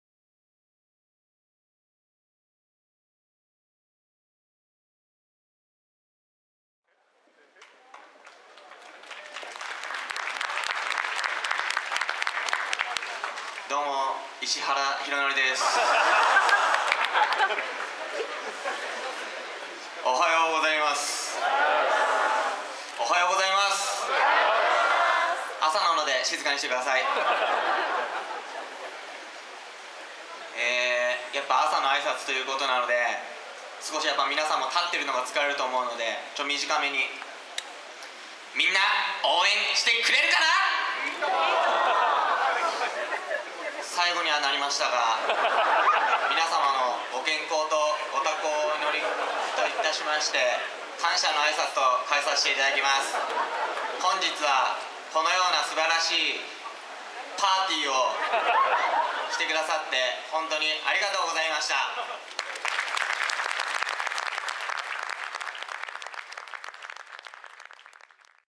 2014 Vegalta仙台 キャンプ出発壮行式（選手代表挨拶：石川大徳）
司会の紹介で名前を「石原」と間違えられそうになったのを、速攻でネタにww